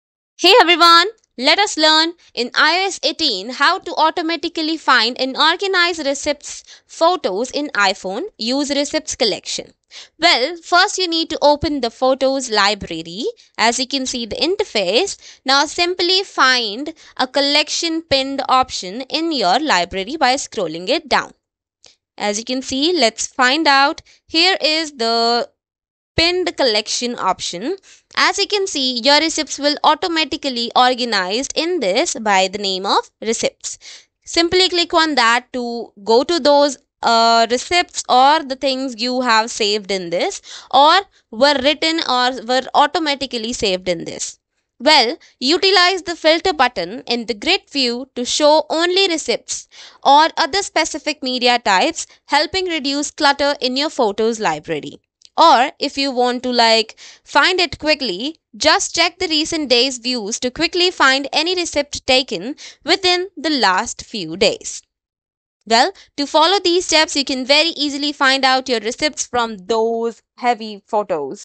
iOS 18: How To Automatically Find And Organize Receipt Photos In iPhone - Use Receipts Collection
In this video, we explore the new capabilities of iOS 18, focusing on the Receipts Collection feature. Learn how to effortlessly locate and organize your receipt photos on your iPhone, making expense management simpler than ever.